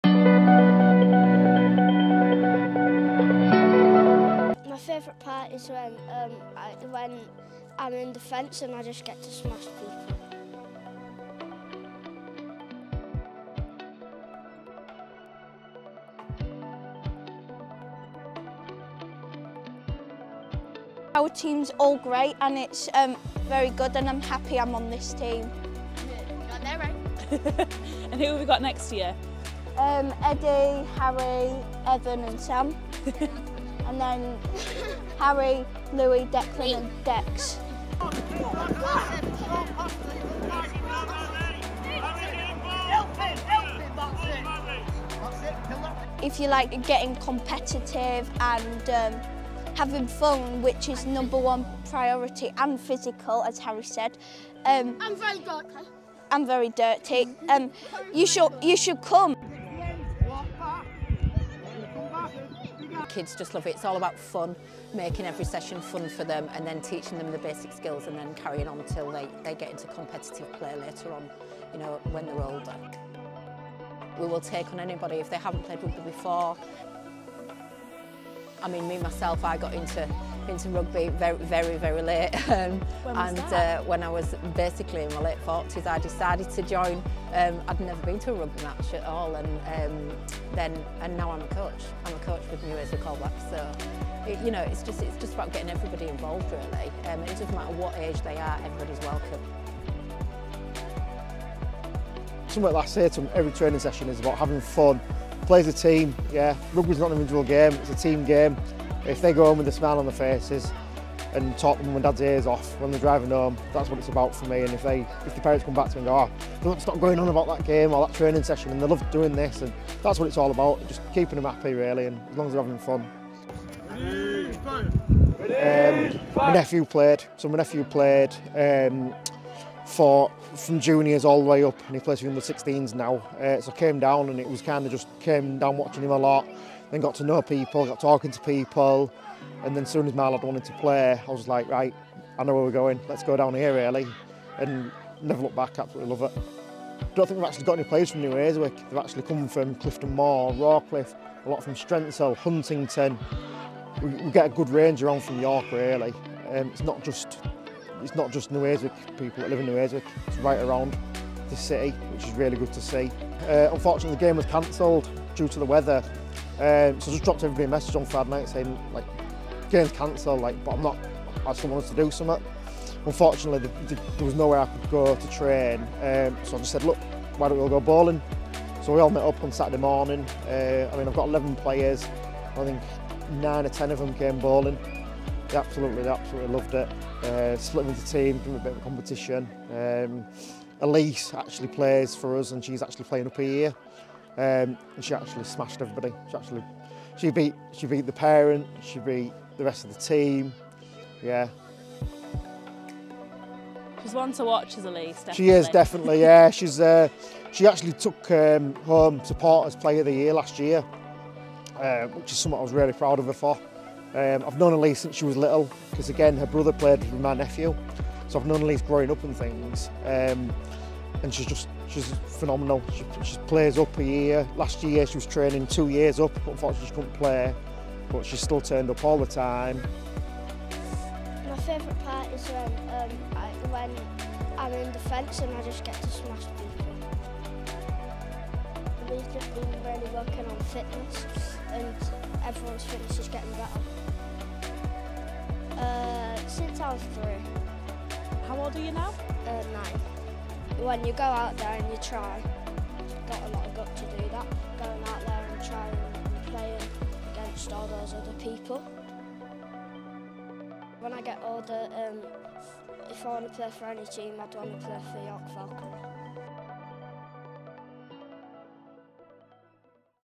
Get Involved in rugby league | New Earswick All Blacks ARLFC
NEAB (New Earswick All Blacks) has over 150 members from ages 5-40 playing rugby on the fields of White Rose Avenue in York's New Earswick village.

Find out what it's like to train with the club and why they think you should Get Involved!